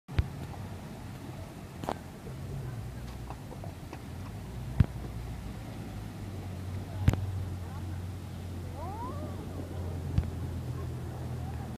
LLUVIA LA PAZ VERNES 25 JULIO DE 2014
Arroyos en cruce de Santos Degollado y Cabildos. Mucha agua.